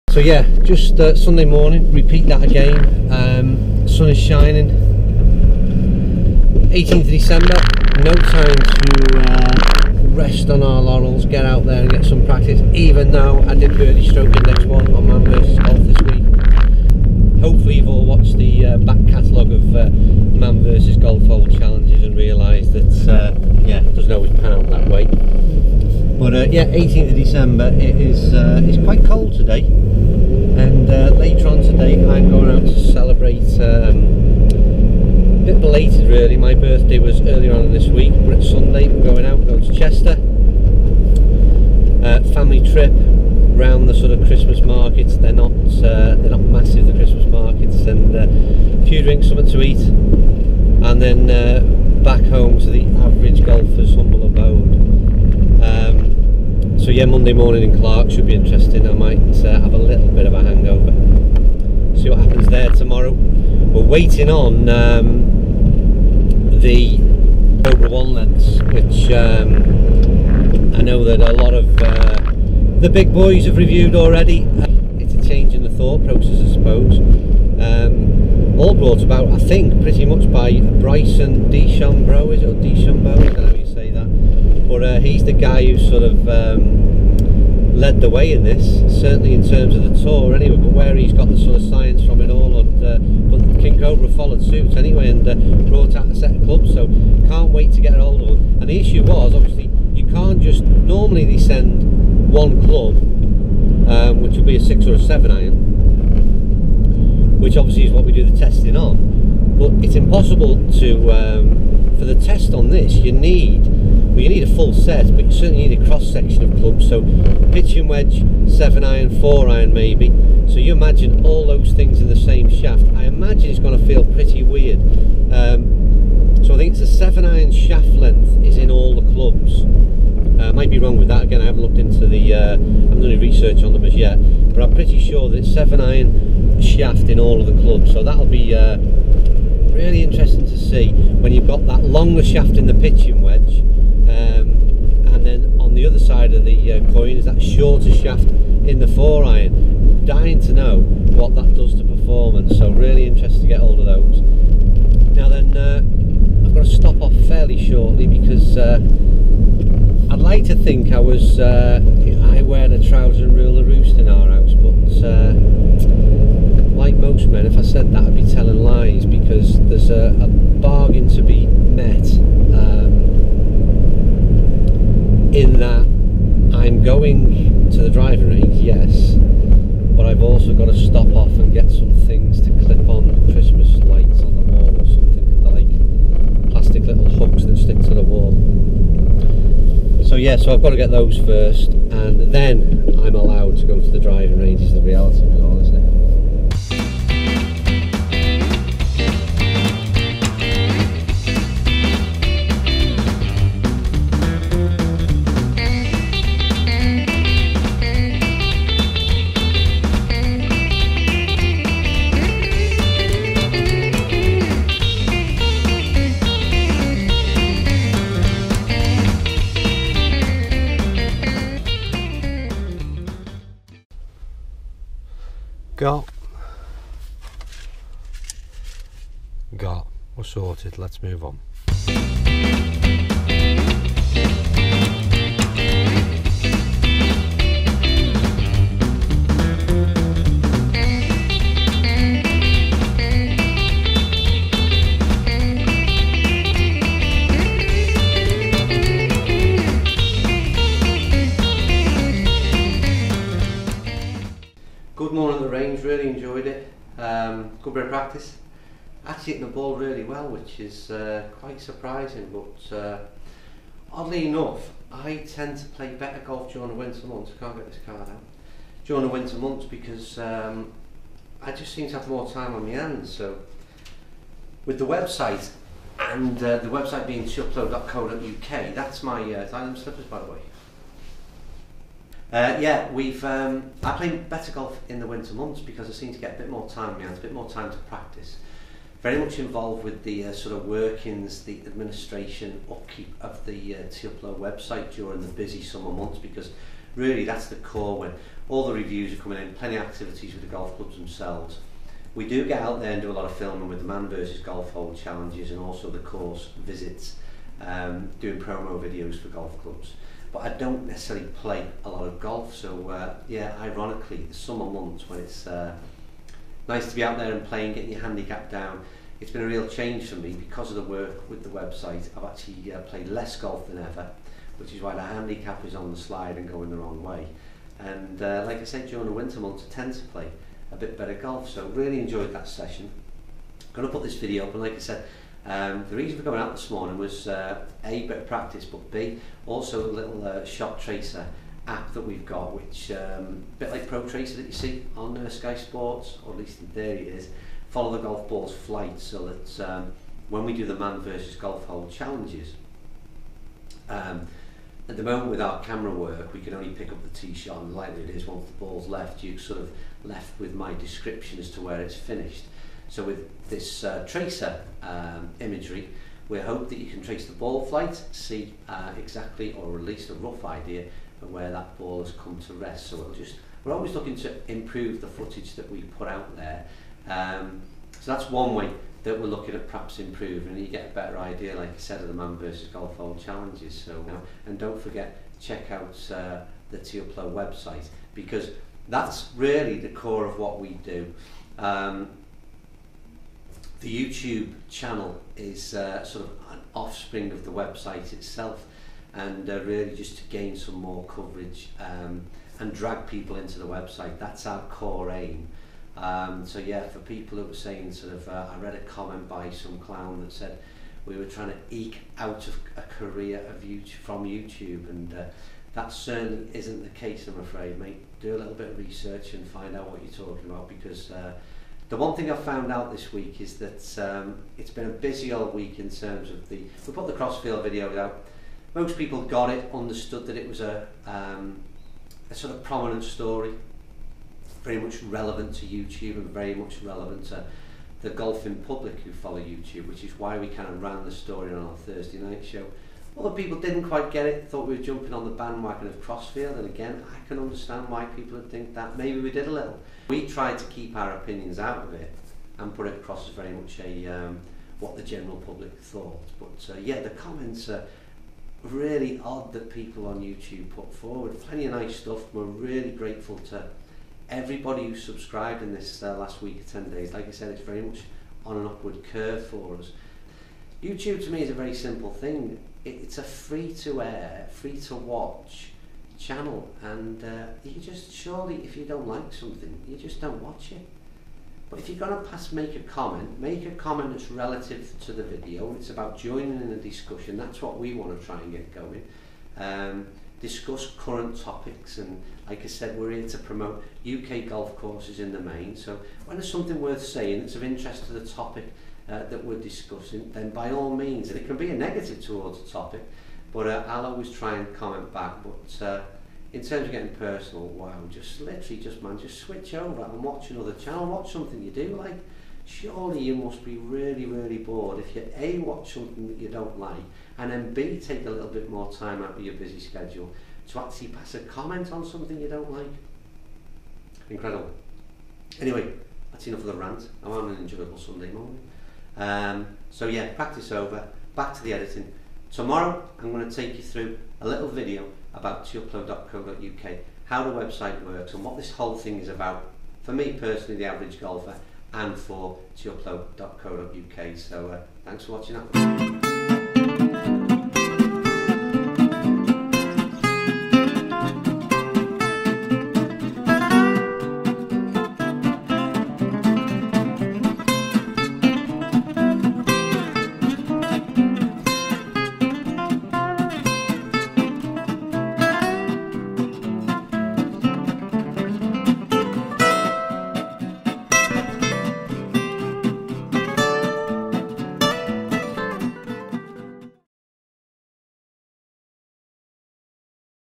Sunday morning with the Average Golfer
Come and take a quick drive with me and Bryson while we discuss the NEW Cobra one length irons while I get ready for my birthday night out.